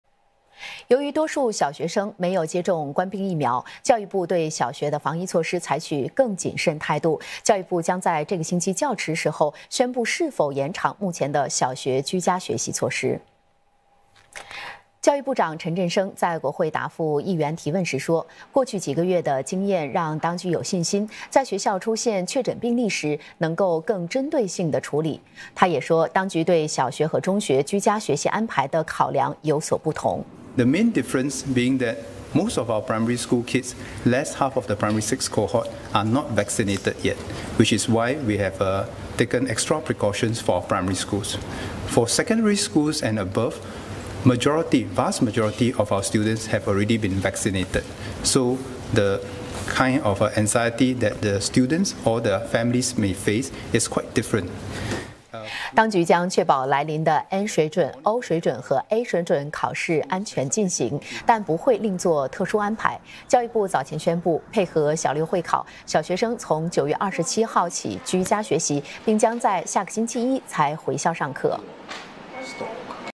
小学生居家学习会否延长 本周内将有宣布
由于多数小学生没有接种冠病疫苗，教育部对小学的防疫措施采取更谨慎态度。